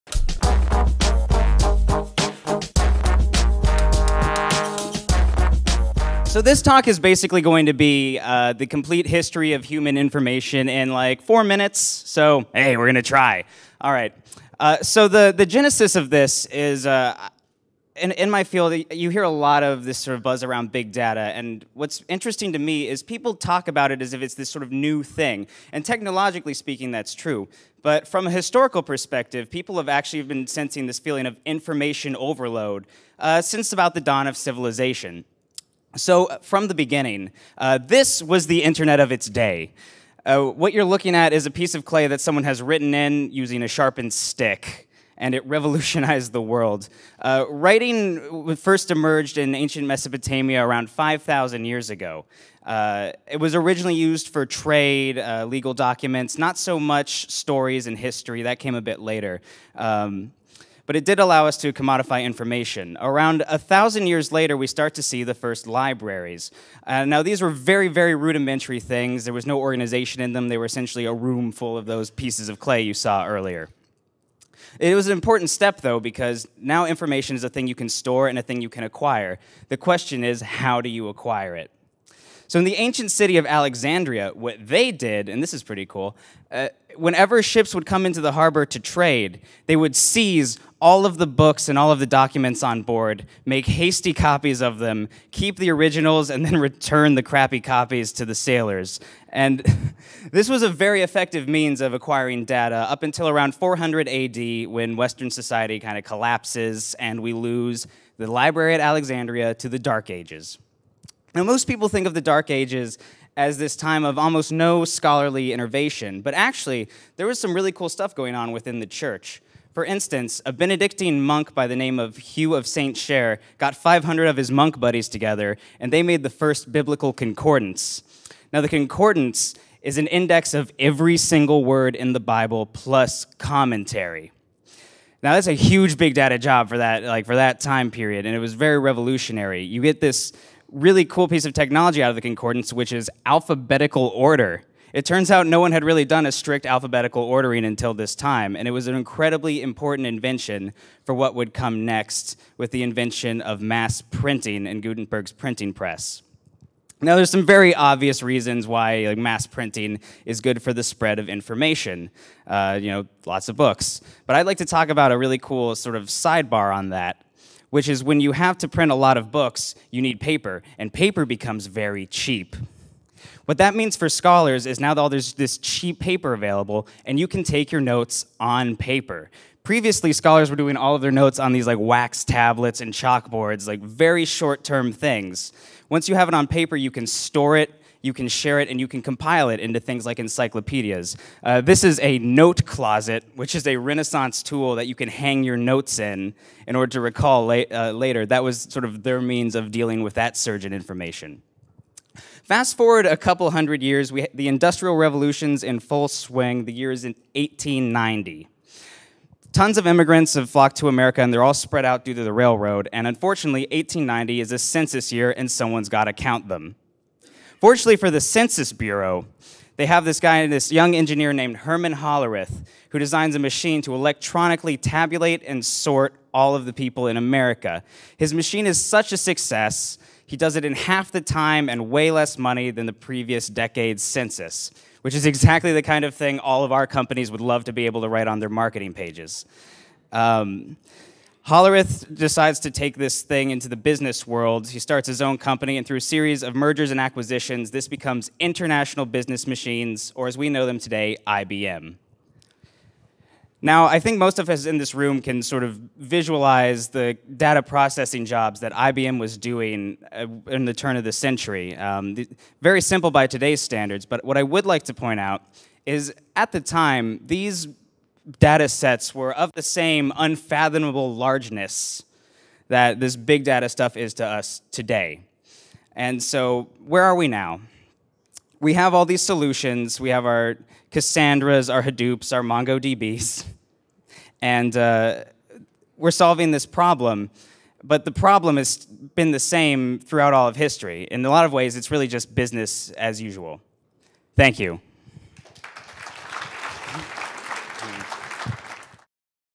Kyle Erf's Ignite talk: "Why Data Has Always Been Big" (Velocity NY 2014)
Every day, an article is published explaining Big Data, our generation’s new struggle to manage more information than it can readily process. But did you know the Ancient Greeks struggled with the very same problem? This talk will present a very brief history of how and why the amount of available information has always outpaced our ability to fully comprehend it.